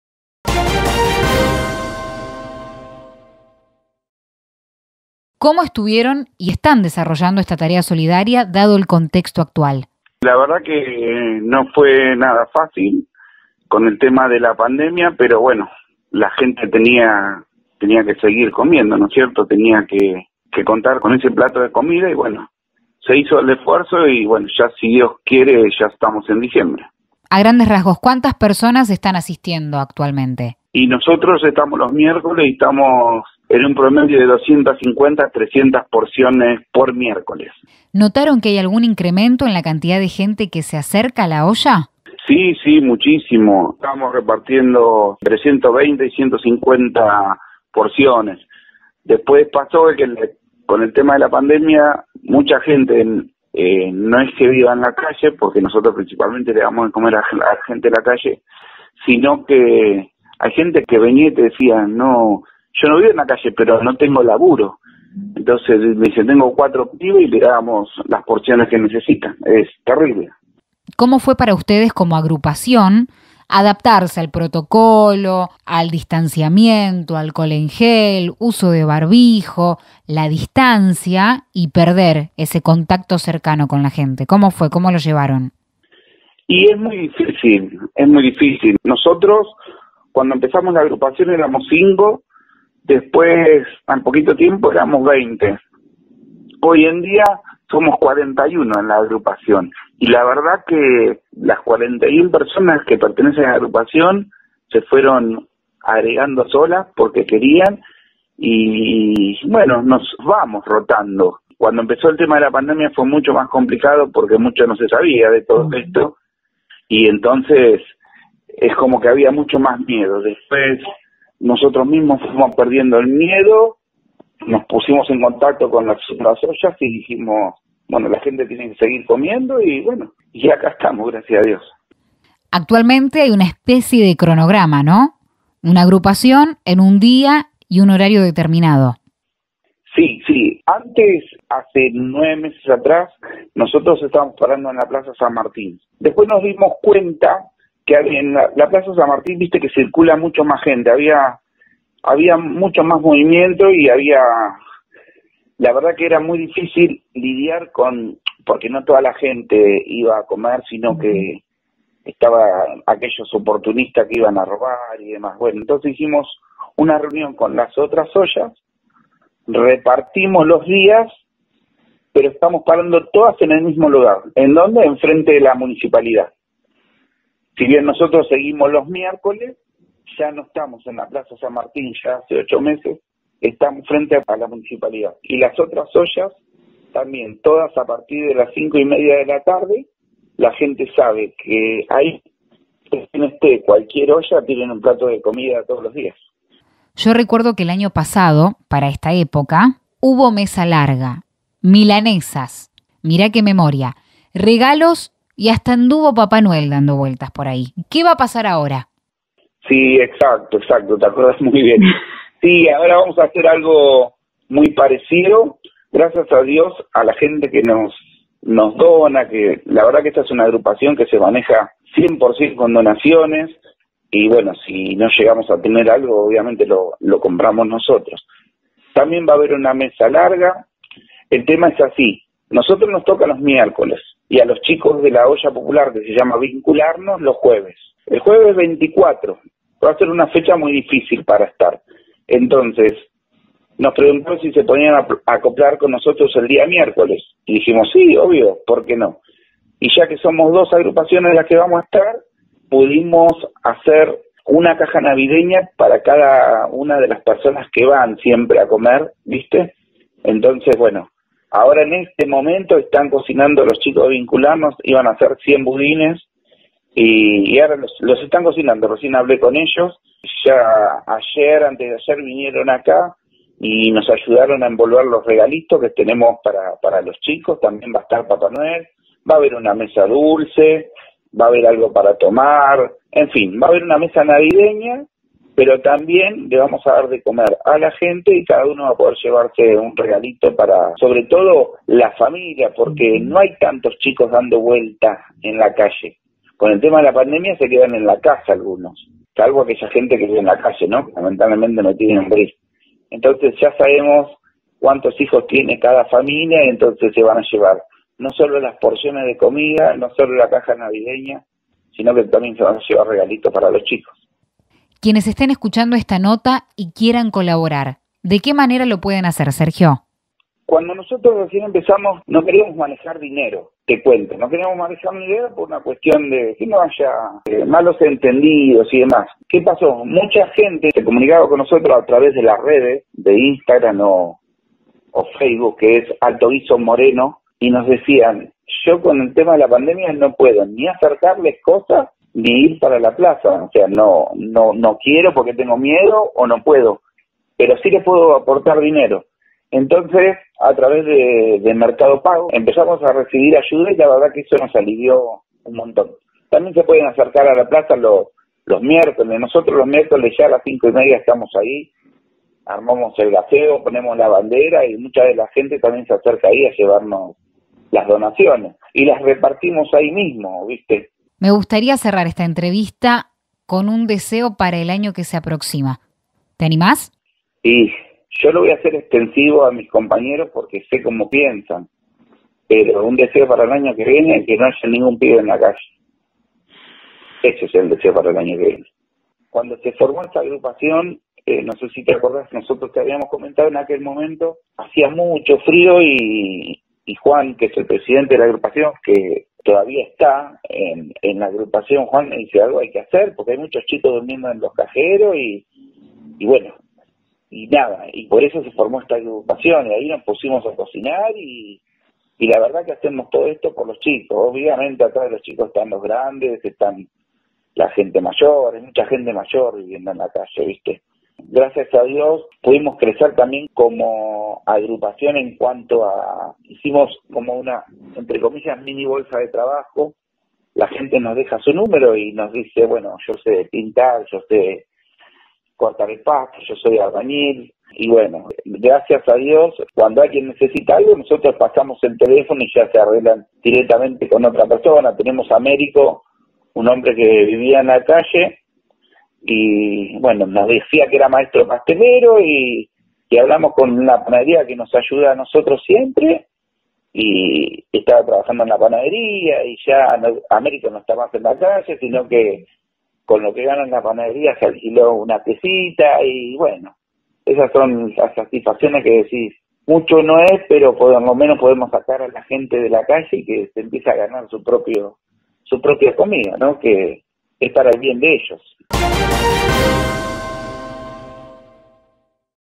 La solidaridad en tiempos de pandemia, con mesa navideña incluida
Entrevista con Sergio Pasqua, integrante de "Alto Guiso"